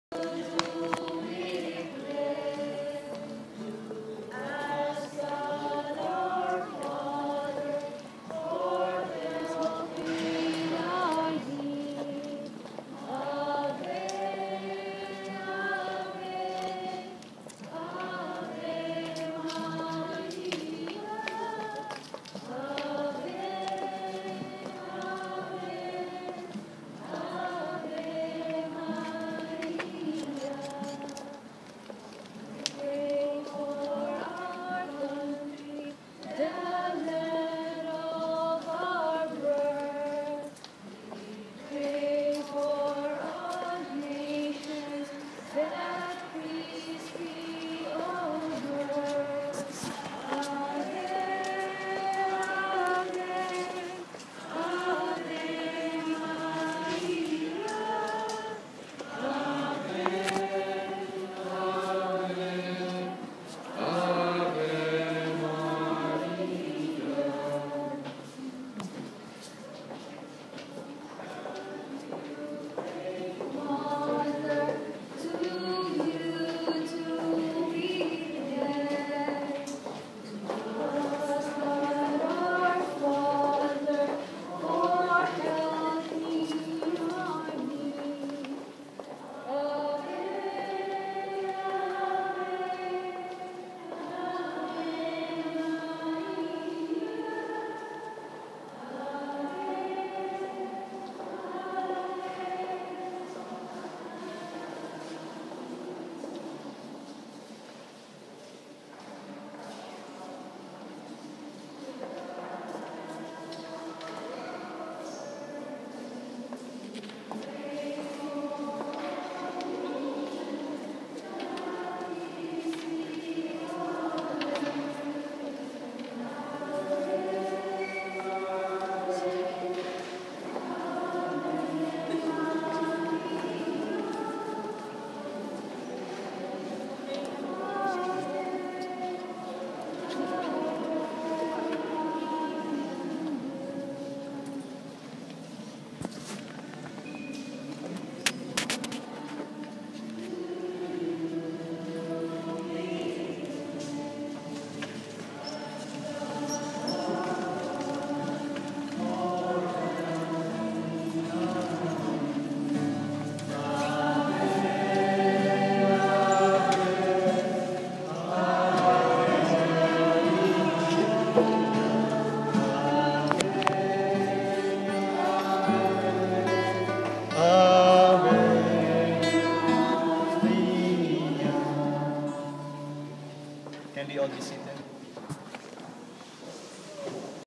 Anniversary of the group that's devoted to the lady of perpetual.
Church activists and other devoted Christians run the virgin Mary feast at the Salesian monestary, Ratisbon Church, Jerusalem. On Wednesday, 2nd of May 2012.

Note from the editor:
It's important to thank the person who first initiated the Wednesday mass on that Church mentioned above was a filipina woman named Miss Asther Paglinawan.